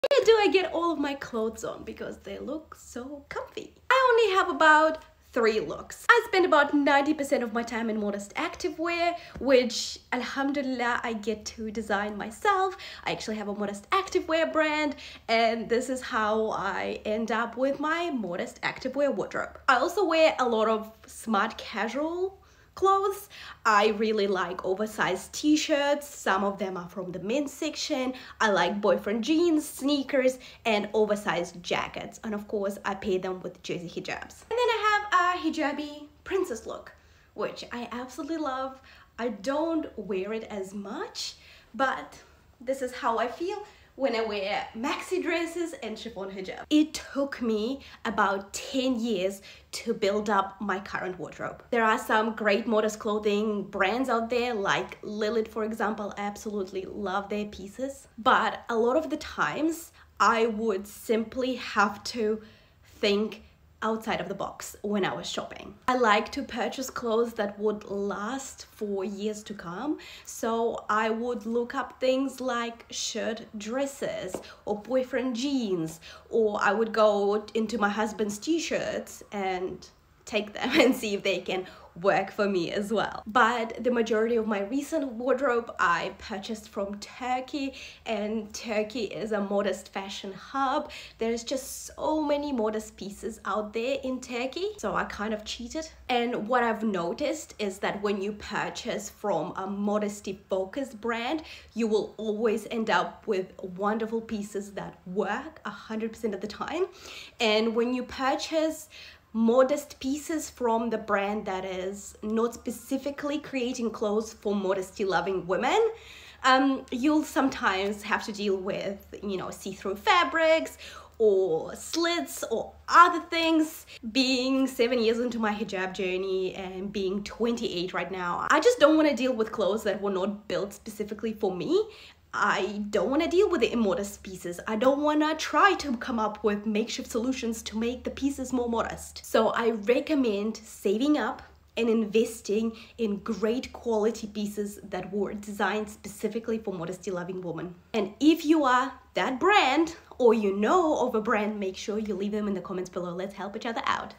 Where I Get my Modest Clothing from As a Muslim Hijabi Woman [ Building Hijab Friendly Wardrobe ]
I want to share with you where I get my modest clothing from as a Muslim hijabi woman! Tune in!